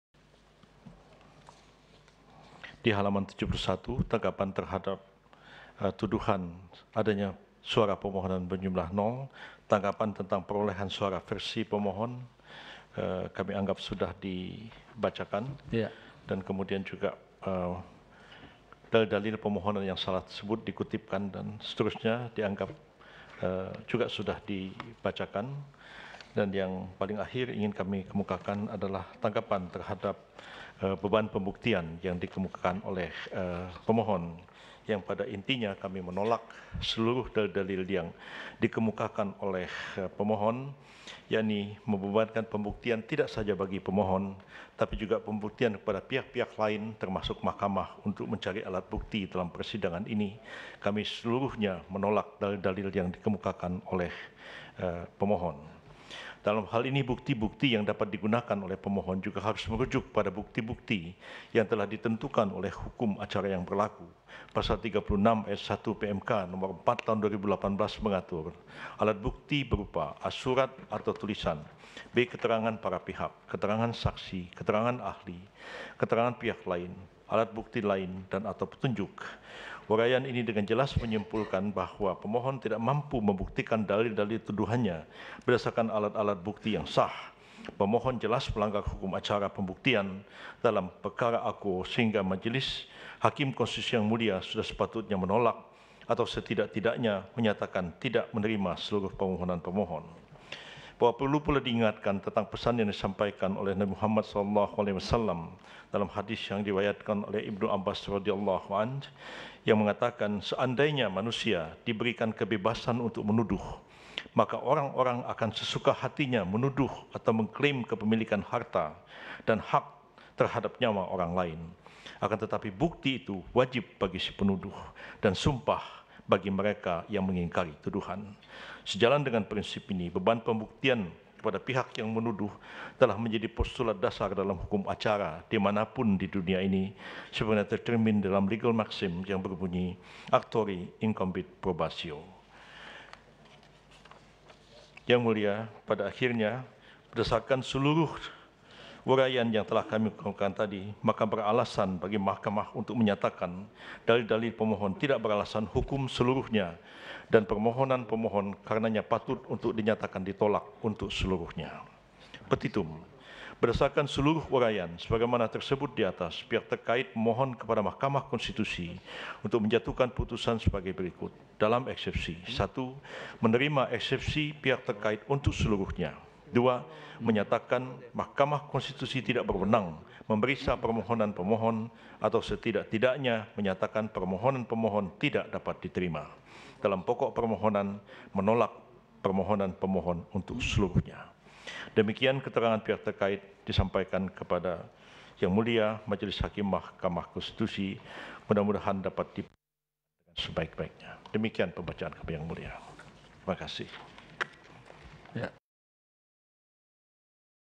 TKN : Memohon MK Menerima Eksepsi Pihak Terkait
Mahkamah Konstitusi (MK) kembali menggelar sidang kedua sengketa Pilpres 2019 dengan agenda mendengarkan jawaban termohon dan  pihak terkait. Yusril Izha Mahendra sebagai ketua kuasa hukum paslon 01 membacakan kesimpulan tanggapan dari pihak terkait (TKN) yaitu menolak seluruh dalil yang di sampaikan BPN serta memohon kepada MK untuk menerima eksepsi pihak terkait untuk seluruhnya, menyatakan MK tidak berwenang memeriksa permohonan pemohon atau menyatakan permohonan pemohon tidak dapat diterima.

Selengkapnya saksikan Breaking News Sidang Sengketa Pilpres 2019, CNBC Indonesia (Selasa, 18/6/2019).